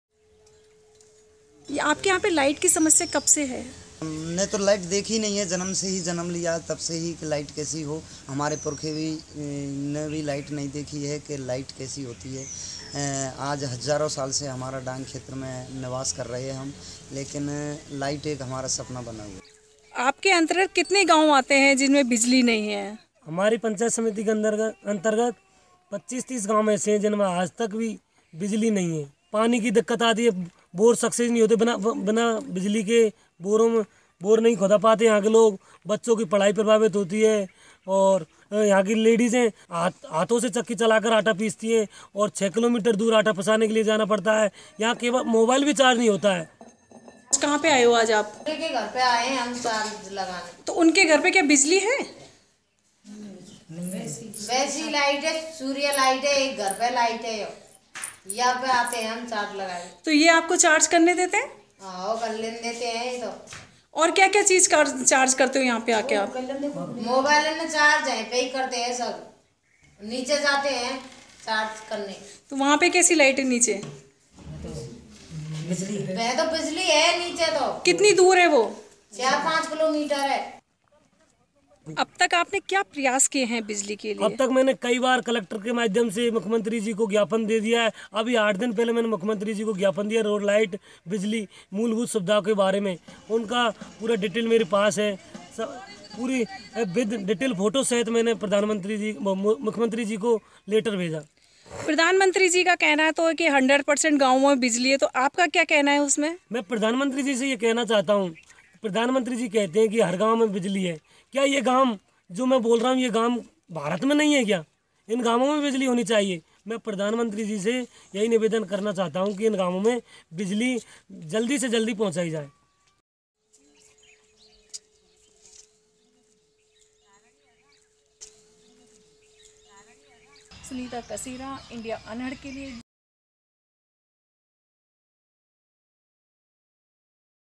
30 Villages In Karauli Don't Have Electricity
Community Correspondent Sunita Kasera) reports from Karauli, Rajasthan for Video Volunteers.